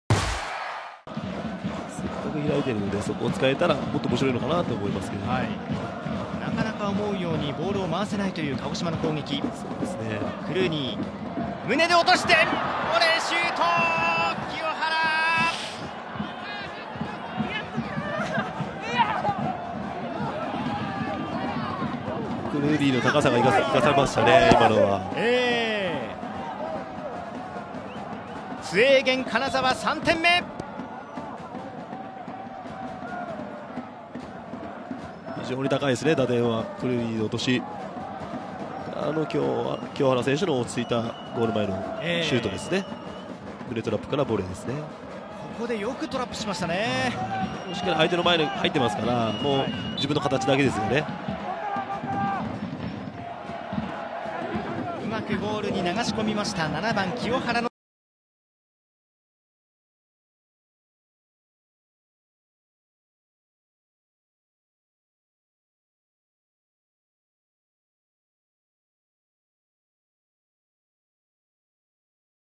【公式】ゴール動画：清原 翔平（金沢）86分 鹿児島ユナイテッドＦＣvsツエーゲン金沢 明治安田生命Ｊ２リーグ 第7節 2019/4/3
ロングボールの落としを胸で収めた清原 翔平（金沢）がボレーシュートを叩き込んで試合を決定付ける追加点を獲得！明治安田Ｊ２のゴール動画を公開中！
2019年4月3日（水）に行われた明治安田生命Ｊ２リーグ 第7節 鹿児島vs金沢のゴール動画です。